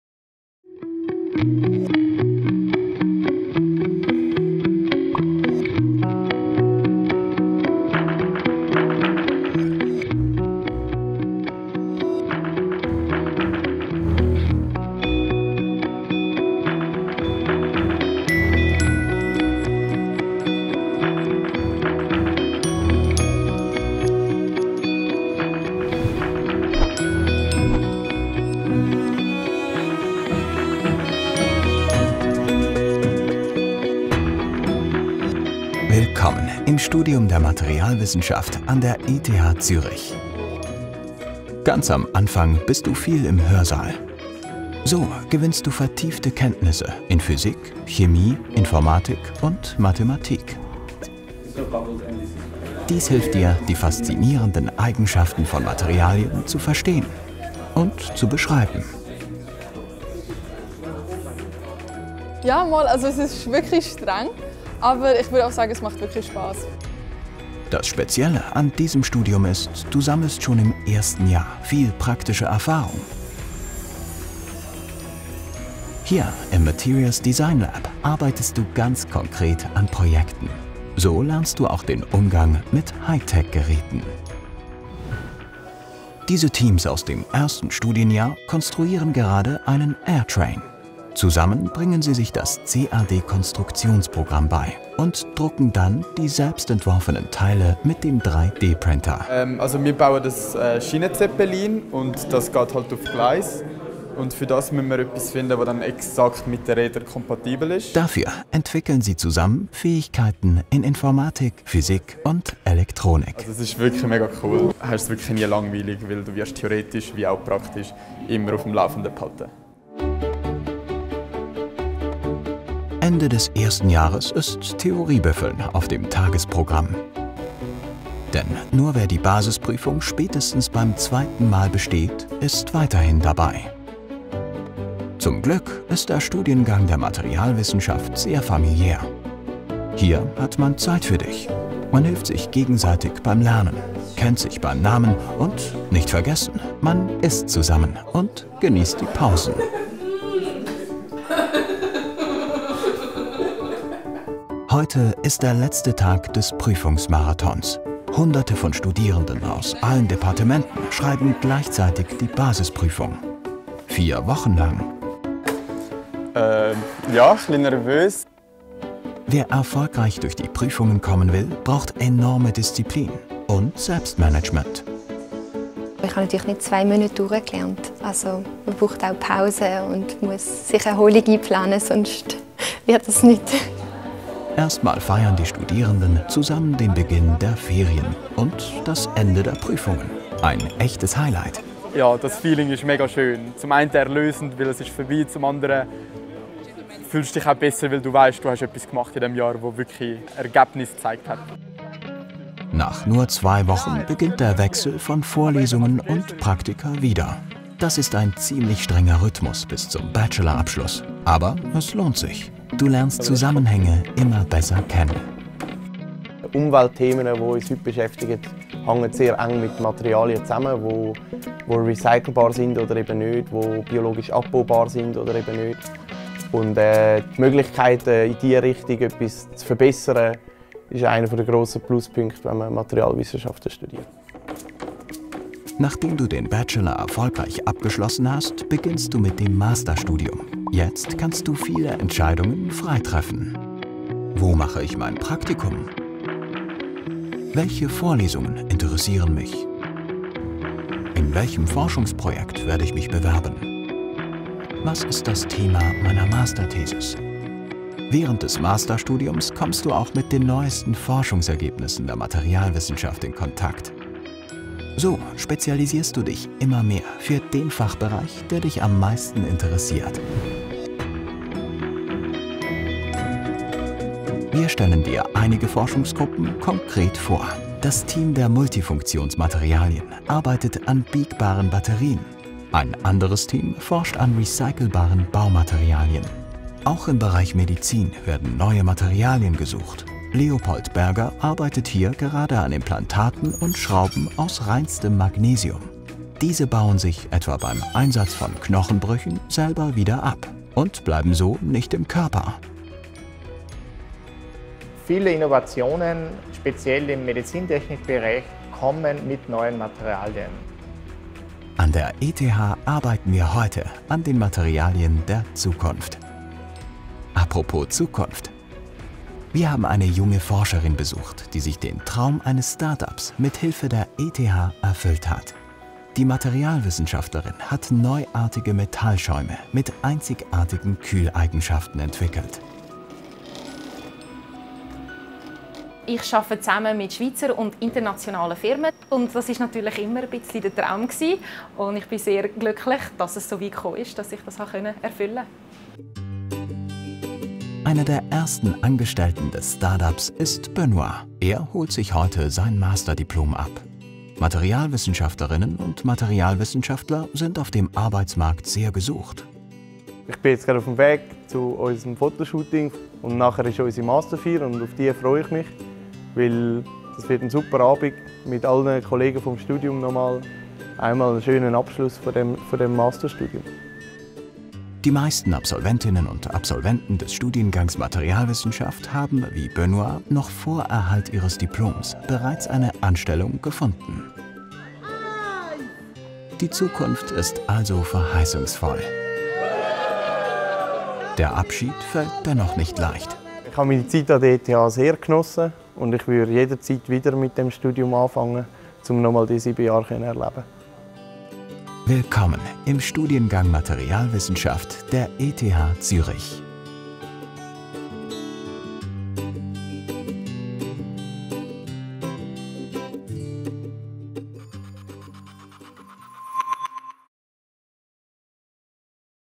Passt das Studium der Materialwissenschaft zu mir?
Wir zeigen dir den Studiengang Materialwissenschaft - vom Bachelor bis zum Master.

Video & Copyright: Dept. Materialwissenschaft ETH Zürich